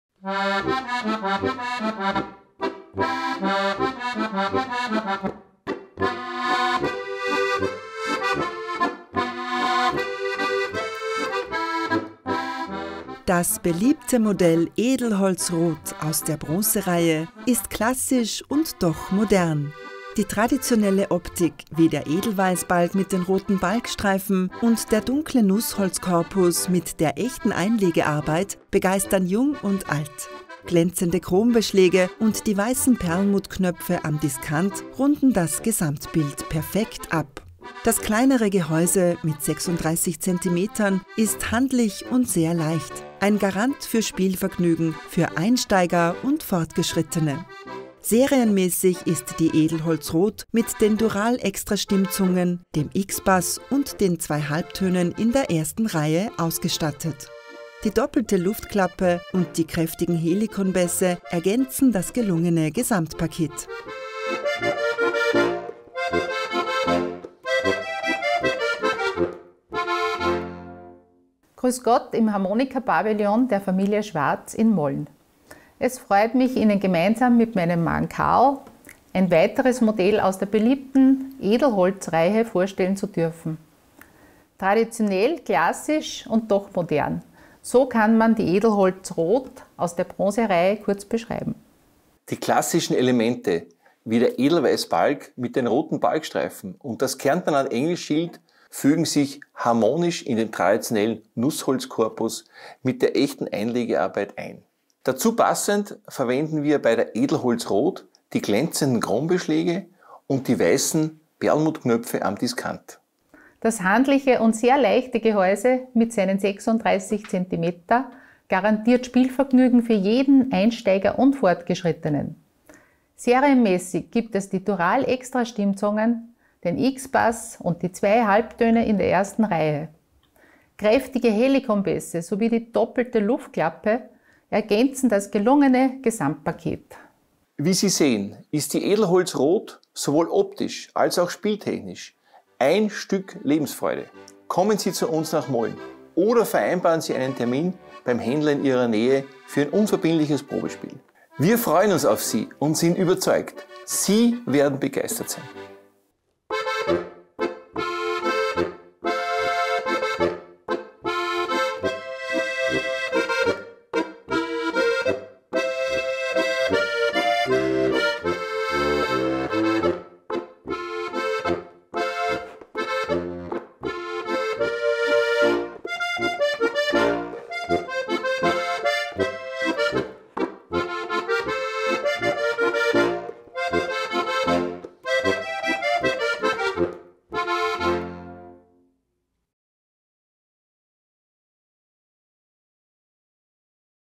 Edelholz rot – Original Kärntnerland Harmonika
Eine ganz traditionelle Harmonika mit klassischen Elementen, wie dem Edelweißbalg mit roten Balgstreifen und dem Kärntnerland-Engelschild.

Die echte Einlegearbeit im glänzenden Nussholz-Korpus verbunden mit den weißen Perlmuttknöpfen fügt sich besonders harmonisch in das Gesamtbild ein.
Das handliche Gehäuse mit nur 36 cm garantiert Spielvergnügen sowohl für Einsteiger als auch Fortgeschrittene.

Auf Wunsch mit PROFI-PAKET (H-Bass + Akkord, Mollbässe auf Zug oder Druck) um nur € 200,-- statt € 620,--!
SIE SPAREN € 420,--!

Serienmäßig perfekt ausgestattet
Dural-Extra Stimmzungen
Basis-Paket = X-Bass und 2 Halbtöne